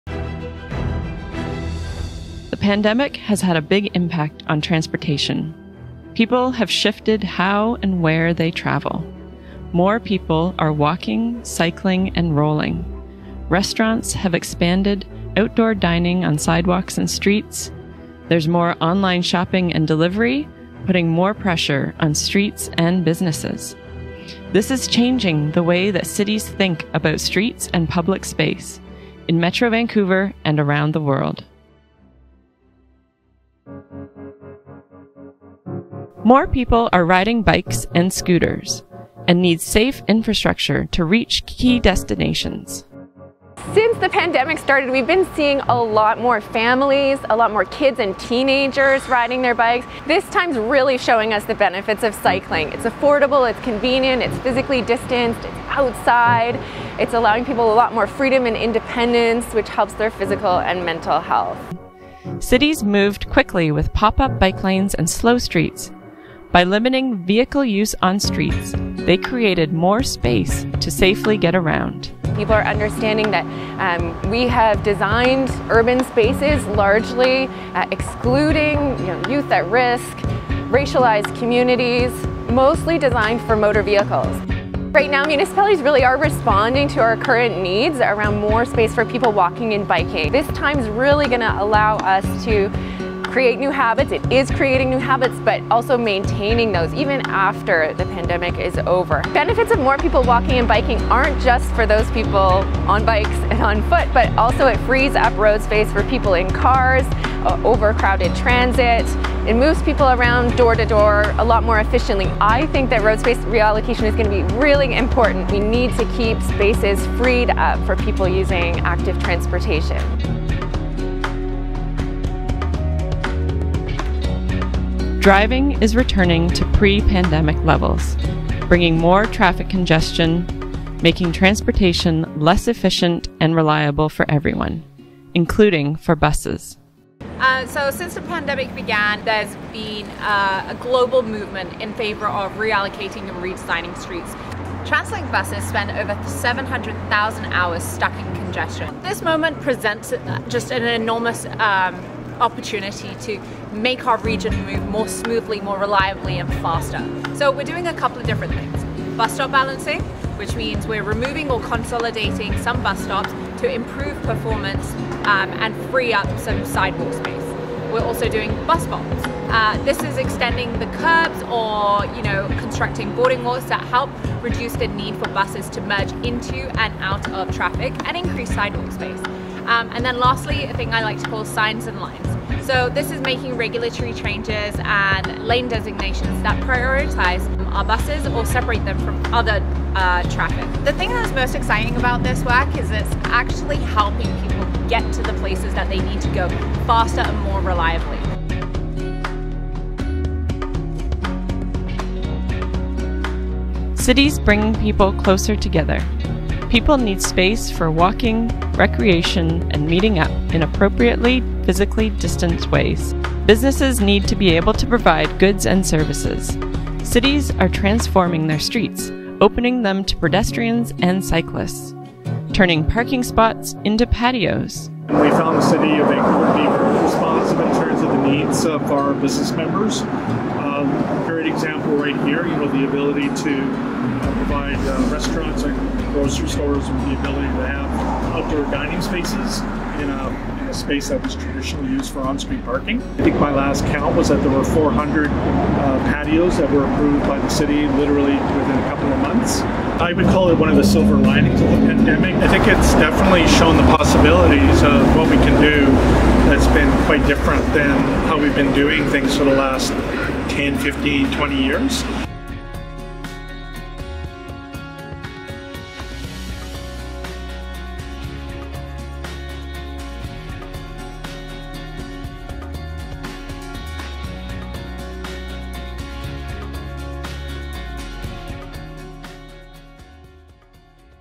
Future of Urban Space and Streets: Bikes, Buses, Pedestrians
The pandemic is challenging cities to reimagine how urban space and streets are used. From tactical bike infrastructure, to bus priority lanes, to repurposing parking, how are cities adapting to the impacts of COVID-19? Will these changes be part of the future of transportation?